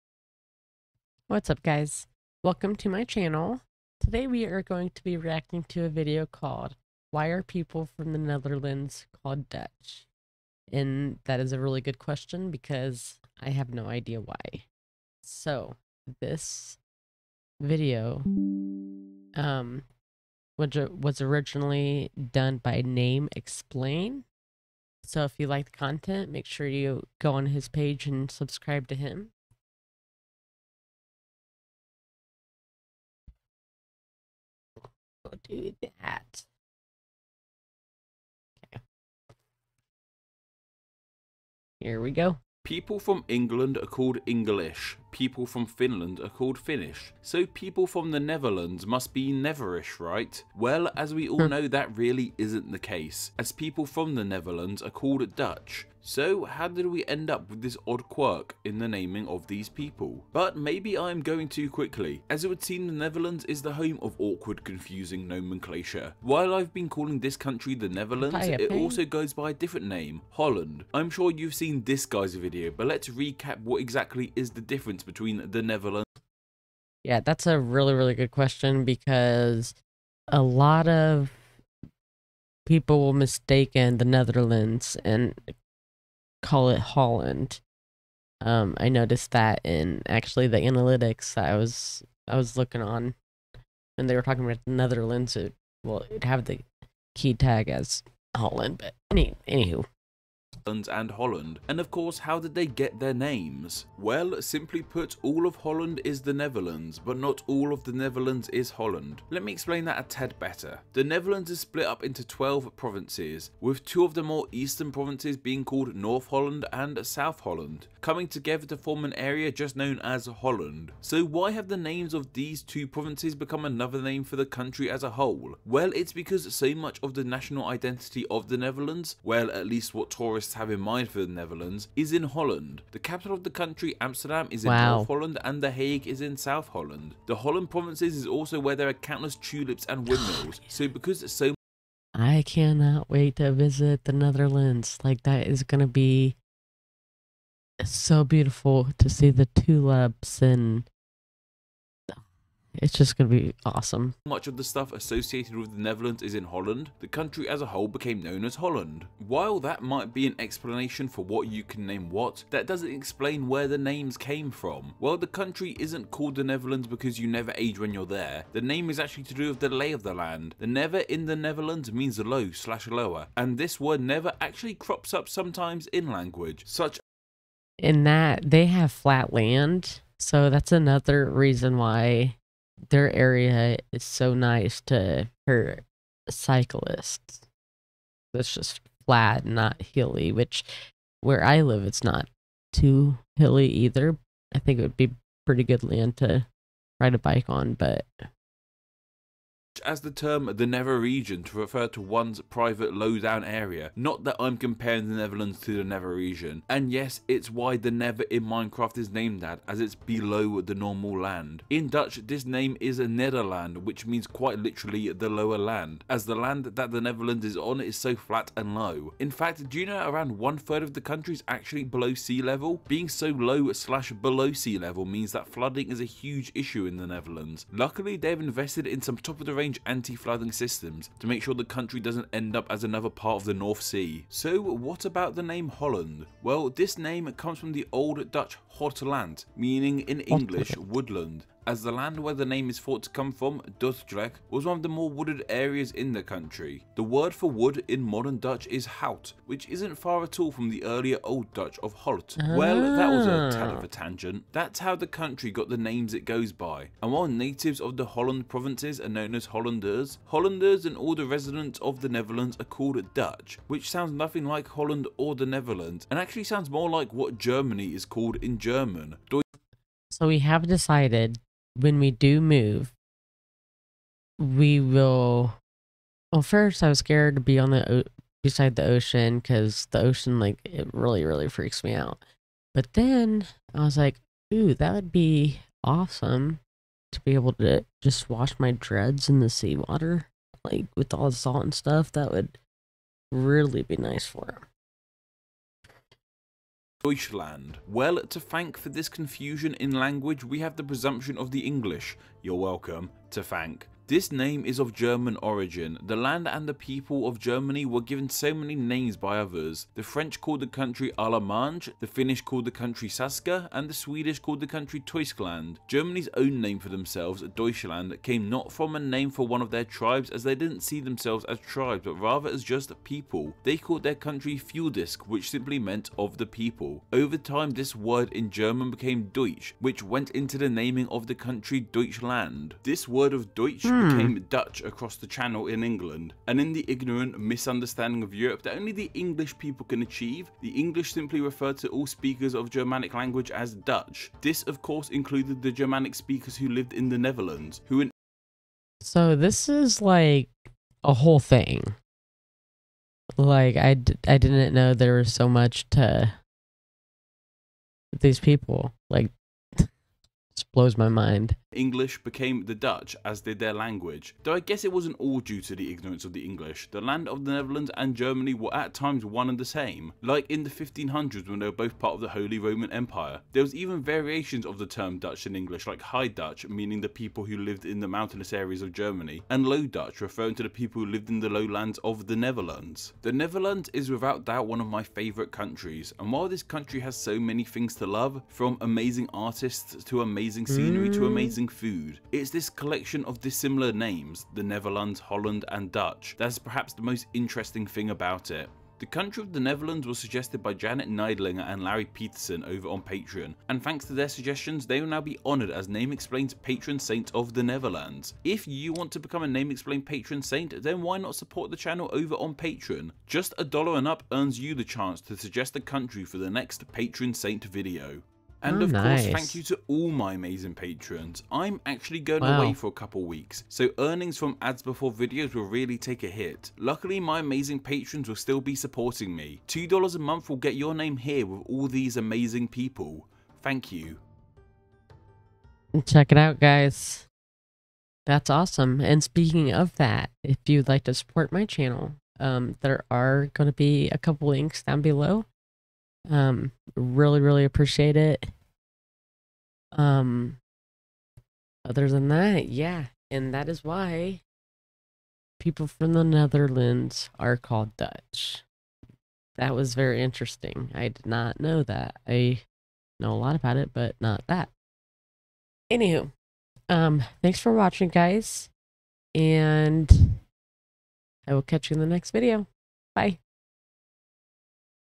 American Reacts to Why Are People From The Netherlands Called Dutch?
American Girl Reacts to Why Are People From The Netherlands Called Dutch?

Thank you for the support and kindness.

purposes such as criticism, comment, news reporting, teaching, scholarship, and